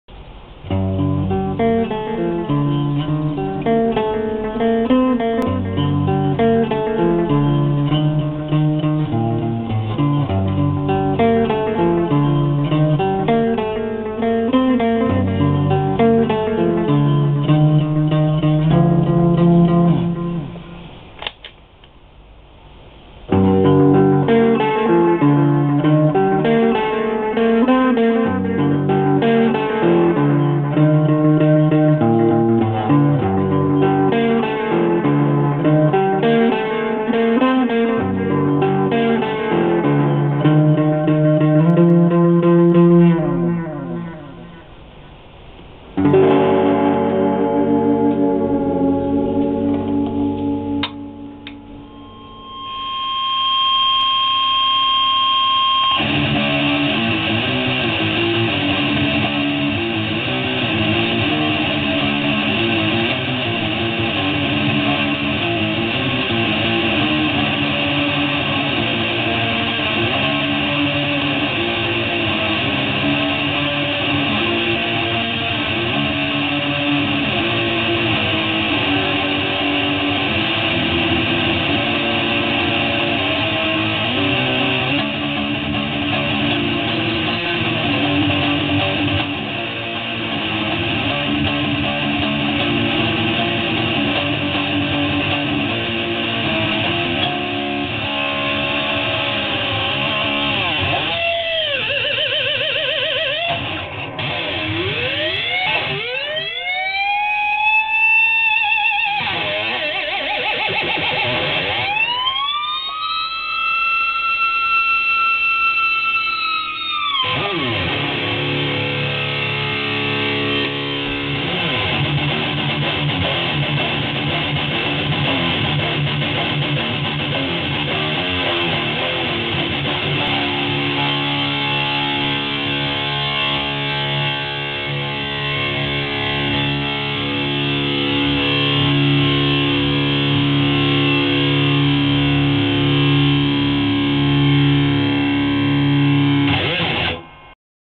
My DBZ Barchetta and effect pedals
Playing around with my new DBZ Barchetta Guitar and some of my pedals. DBZ Barchetta - Boss FV50 Foot Volume, Ibanez Aw7, Apezila Fetz Smooth Rock, Biyang Metal End King, Sabboy Custom Analog Delay - Kustom KG112FX